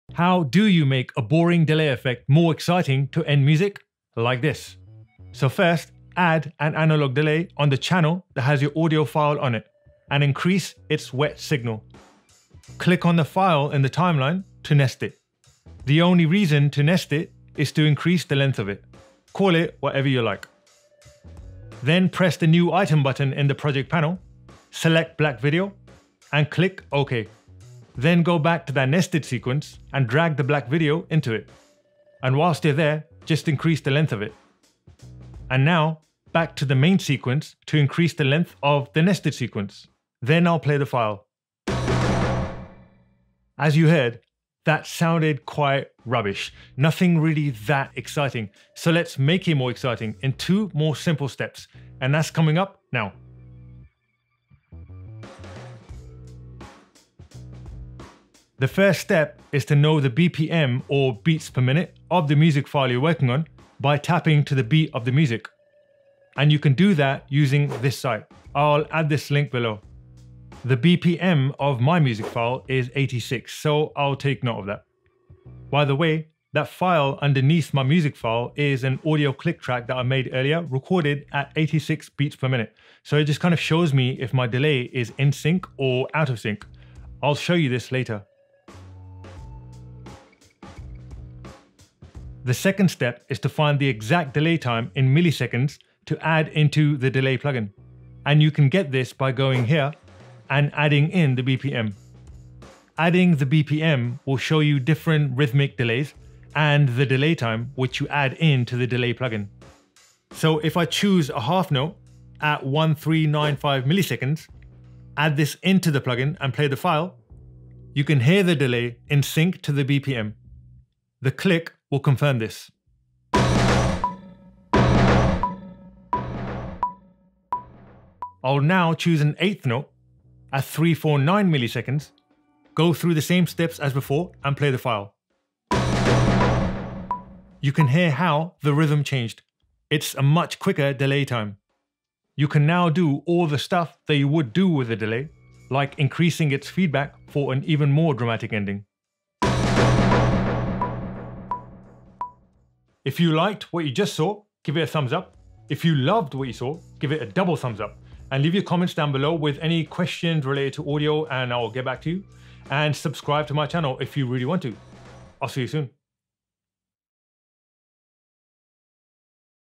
How to End Music with a Delay Effect in Premiere Pro CC 🎵 🎵 🎵
In this video I show how to End Music with a Delay effect BUT ALSO.

I’ll show you:
1. How to find the BPM 🥁  of your Music

AND

2. Find the exact DELAY TIME ⏱  needed for your Music to dance with excitement!

Right, enough reading. Press play!

Websites used in Video:

🎶  Tap BPM - Online Beats Per Minute Calculator and Counter

📷  EQUIPMENT / SOFTWARE USED IN THIS VIDEO:

Sony Alpha A6500 Compact System Camera

Commlite AF Auto Focus Mount Adapter